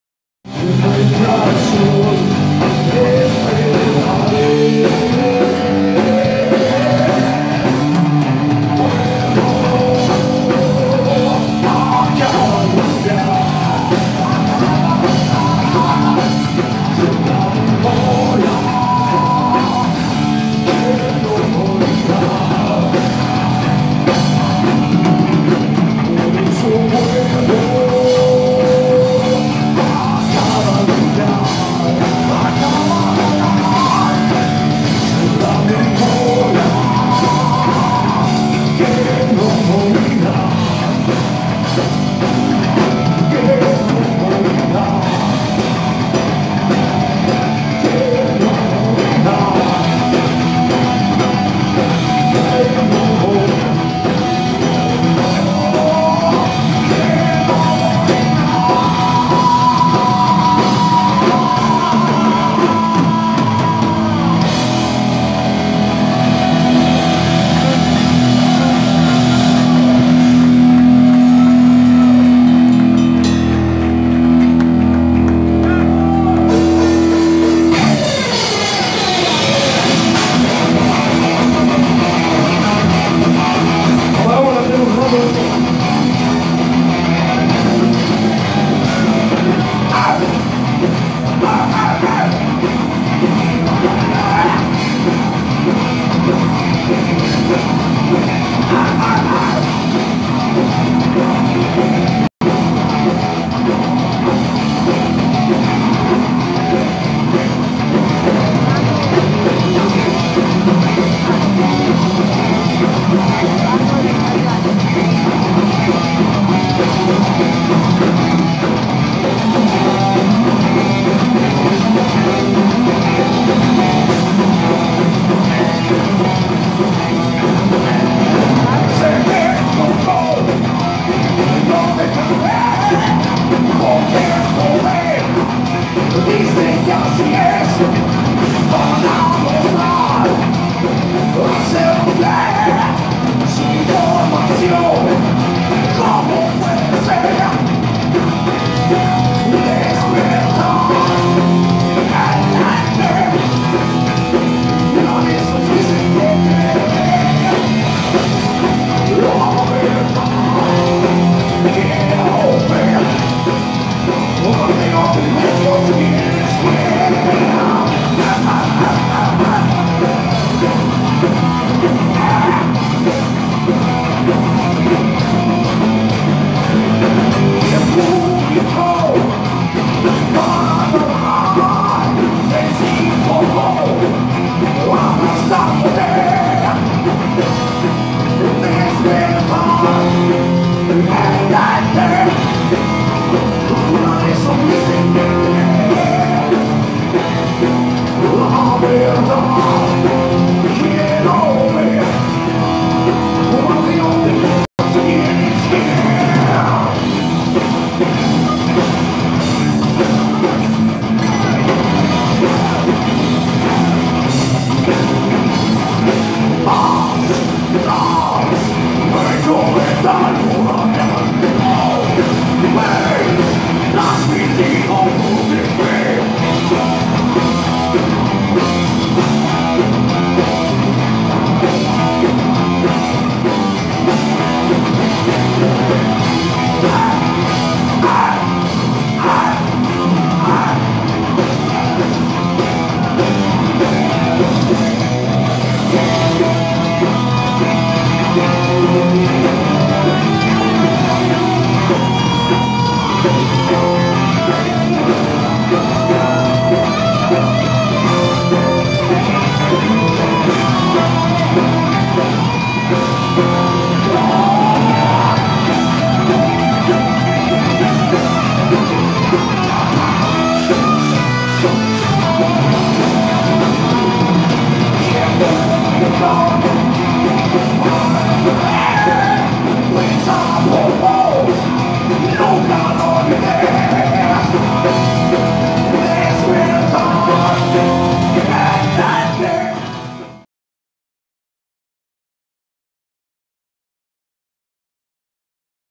TANO ROMANO EN MARQUEE-COBERTURA HONESTIDAD BRUTAL RADIO (13/12/18)
Tano Romano se presentó en El Marquee el 13/12/18 y Honestidad Brutal Radio estuvo ahi.